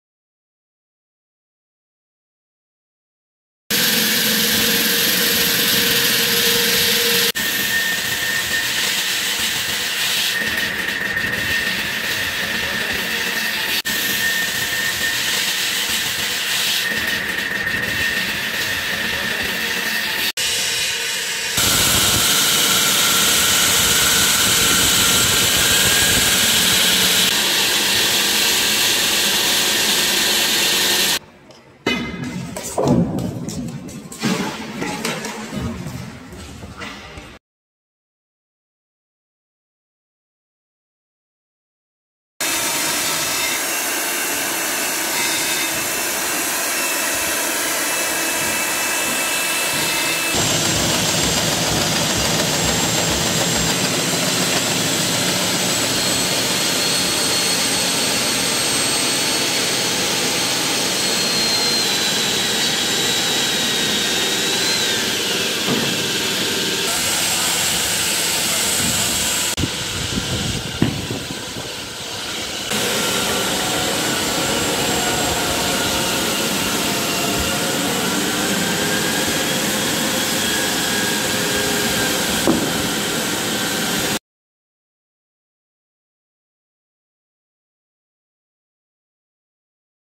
RCC Concrete Cutting with Giant Machines | Core Cutting Machine & Giant floor Saw |Industrial Work
Huge concrete Cutter machine
working of Giant floor Saw & Core Cutting Machine